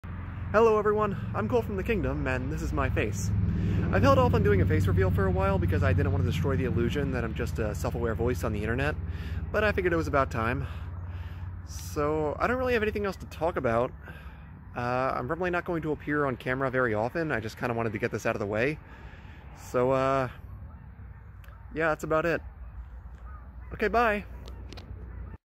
A Moment with Cole - Face Reveal
Just wanted to get my first appearance on camera. I did a few takes of this and it took me way too long to settle on this one. So... hope you enjoy my face!